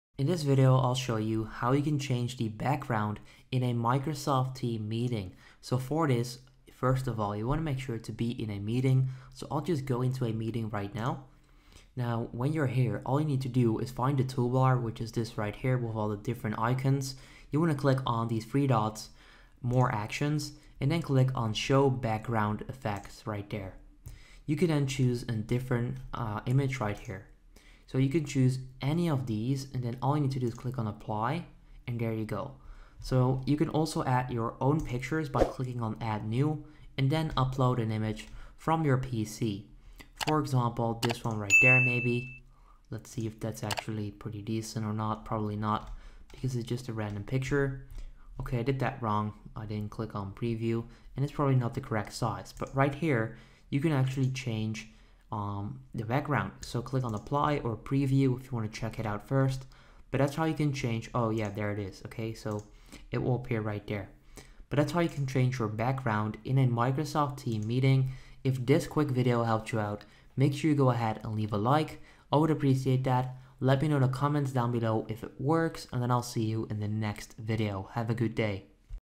How to Change Background in Microsoft Teams Meeting!
Learn How to Change Background in Microsoft Teams Meeting

In this video I show you how you can get a virtual background in a Microsoft teams meeting! So if you want to change your background in teams to a virtual background this is the right video for you!